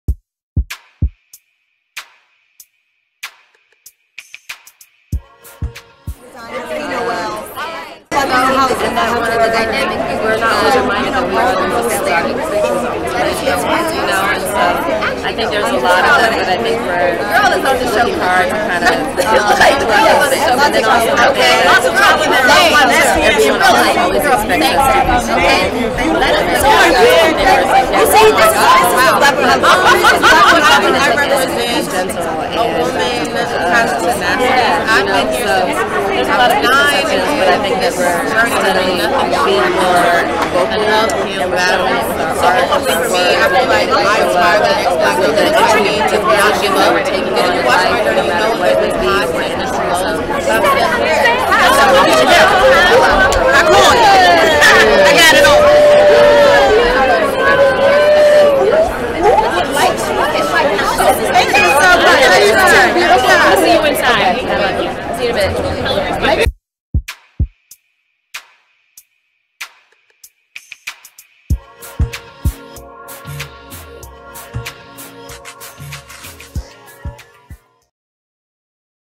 All Access: Bel-Air's CoCo Jones at the Black Women in Hollywood Gold Carpet Event
CoCo Jones gets honest about why she is a Woman for the Culture.

Visit womenfortheculture.com for the latest news on your favorite celebrity news and more.